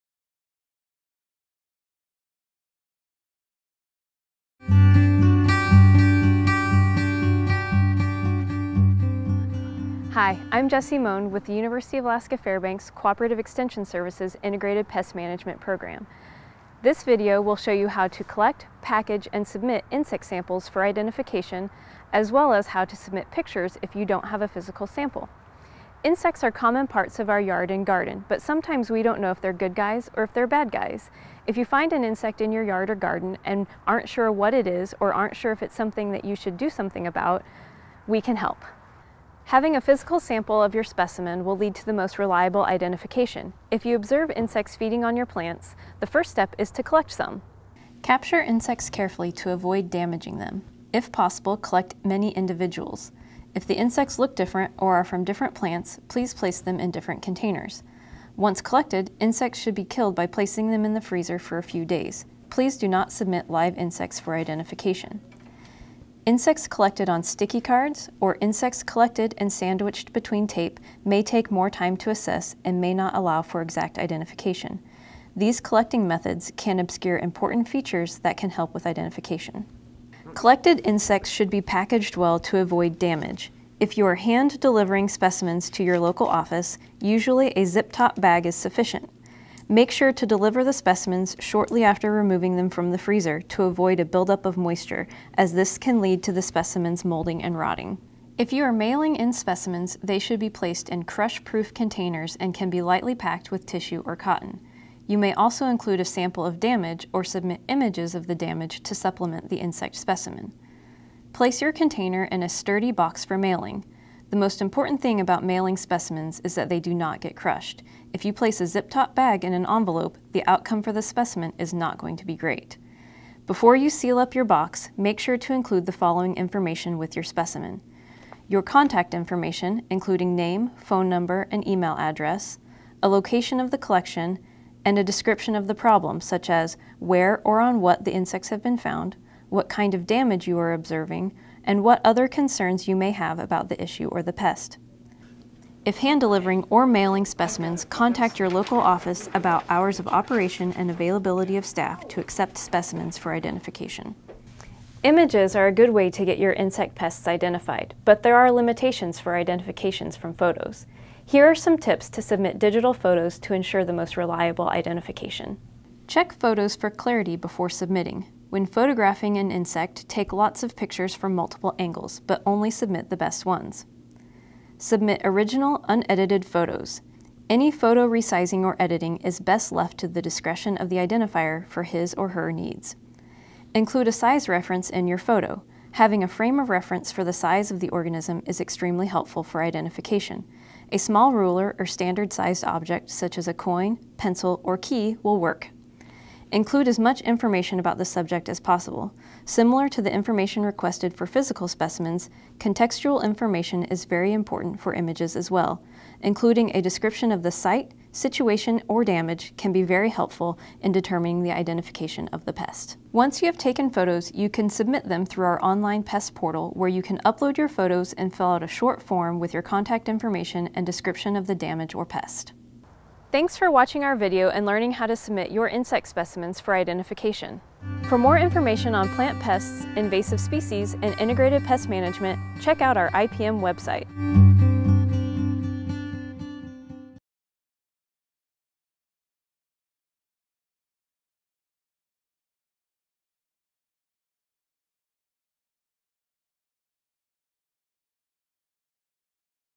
Submitting Insect Samples for Identification
Join UAF Cooperative Extension integrated pest management technician Jessie Moan as she shows the process for submitting insect samples for identification.